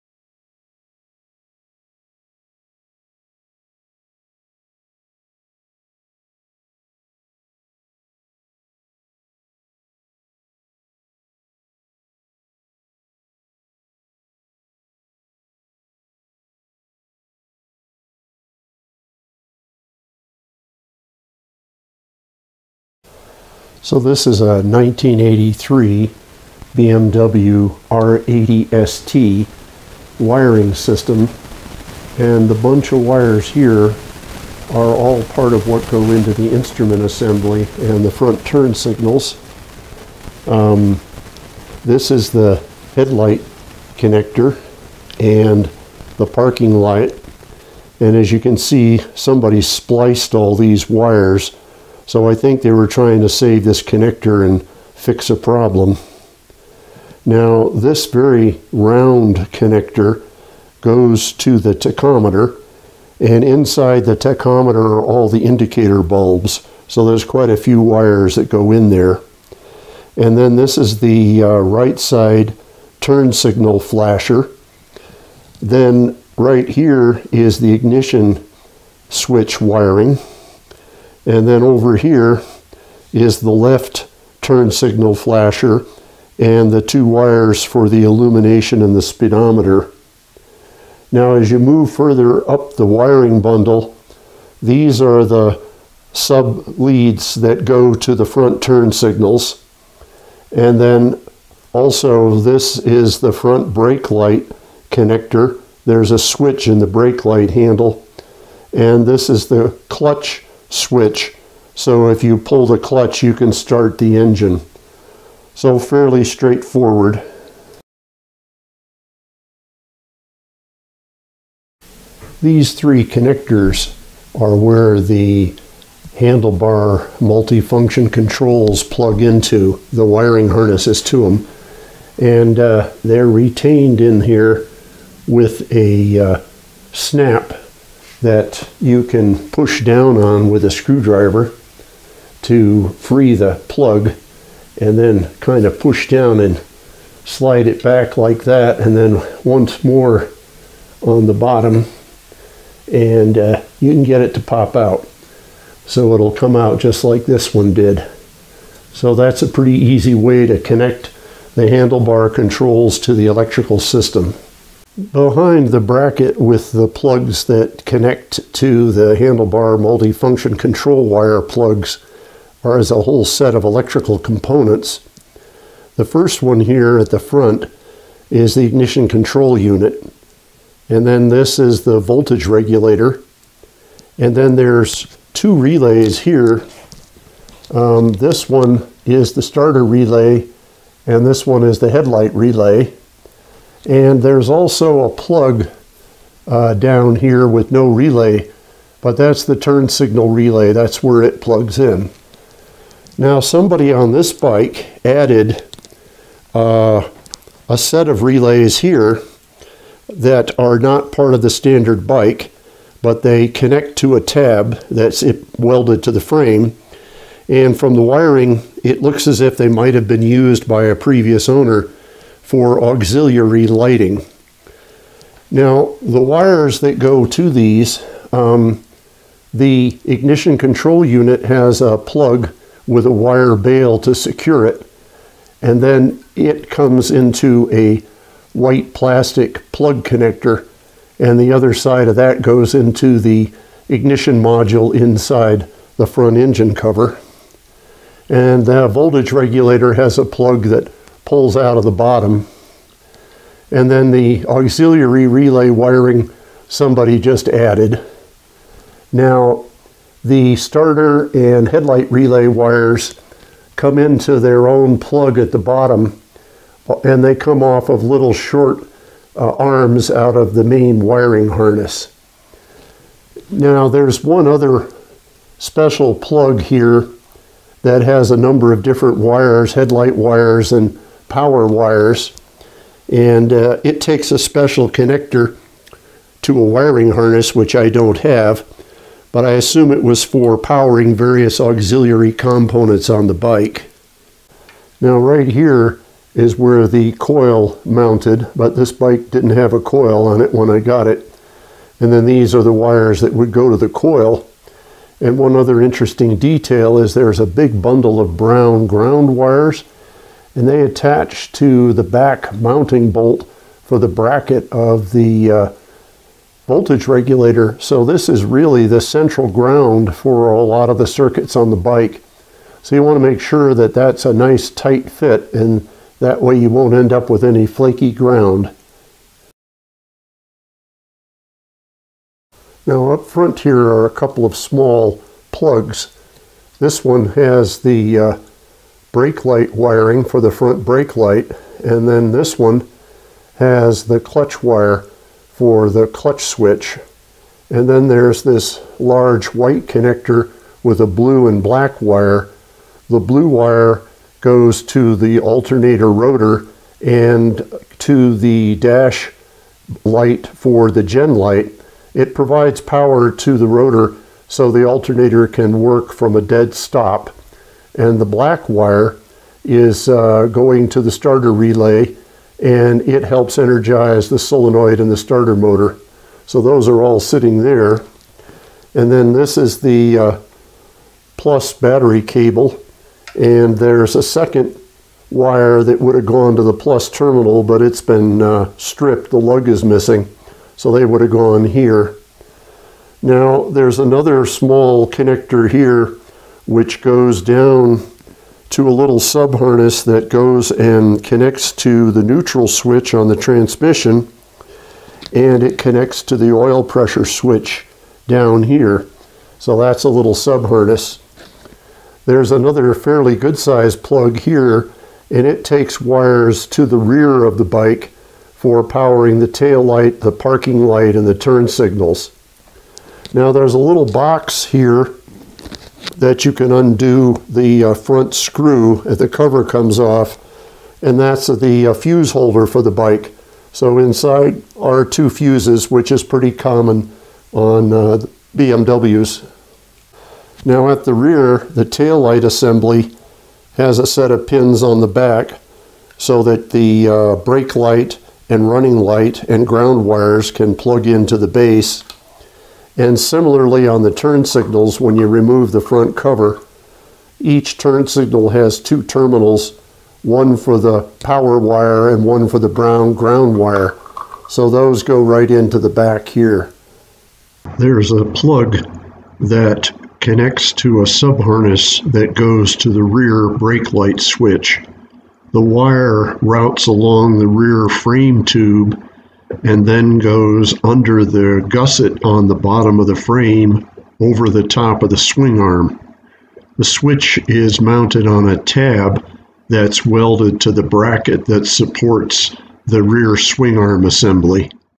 1983 BMW R80ST Electrical System Overview
I show the main components of the R80ST electrical system. Click this link to see how I removed the electrical system.